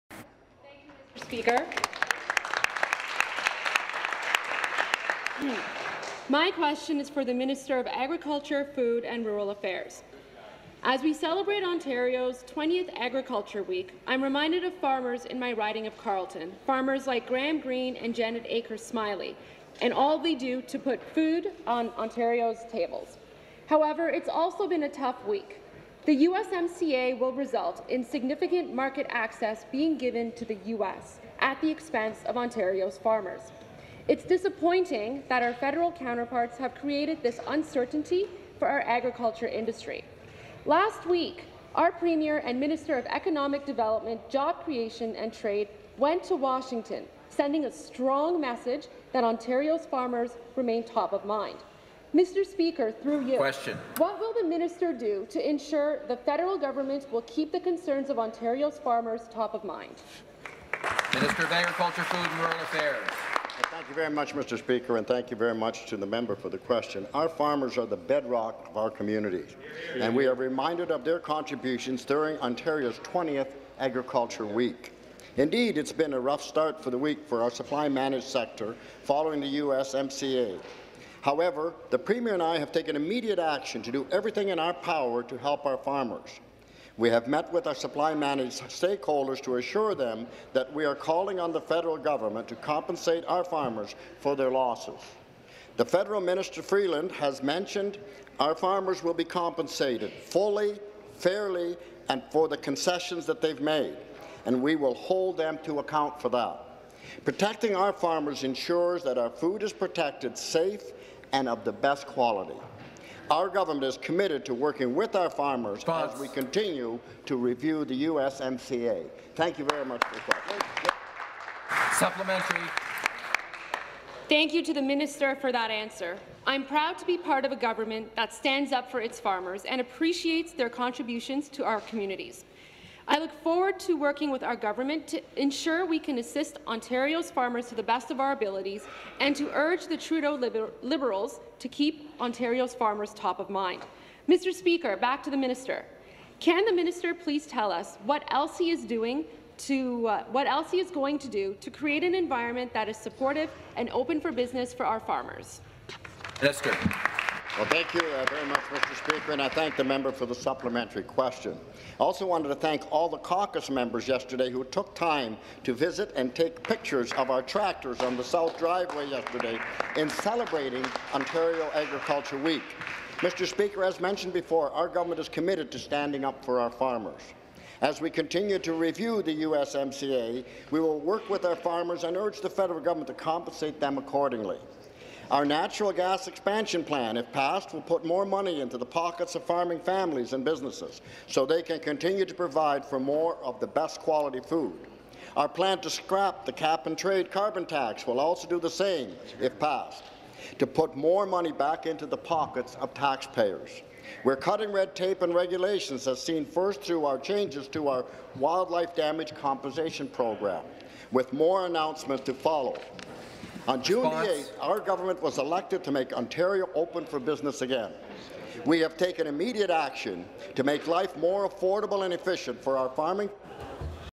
Question Period - Agriculture and the USMCA
On October 3, 2018, Goldie Ghamari asked the Hon. Ernie Hardeman, Minister of Agriculture, Food and Rural Affairs, about how the government plans on supporting Ontario's farmers and voicing their concerns regarding the United States, Mexico and Canada Agreement to the federal government.